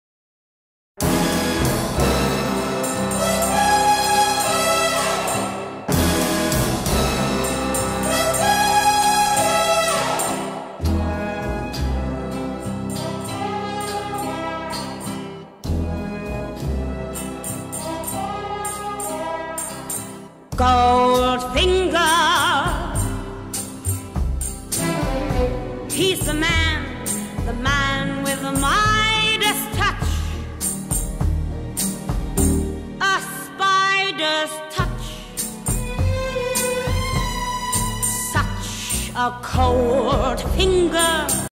Gold Finger, Not the movie, the cocktail ❤️🍸#007
There is nothing villainous about this cocktail, but put these ingredients together and you will think you have the Midas Touch.  Enjoy!

Ingredients
2 oz Orange Vodka
.5 oz Cointreau
.5 oz fresh Orange juice
Orange twist for garnish

Directions
Pour ingredients into a Shaker, add ice and shake to chill. Double strain into a chilled martini glass. Express Orange peel over glass and use twist to garnish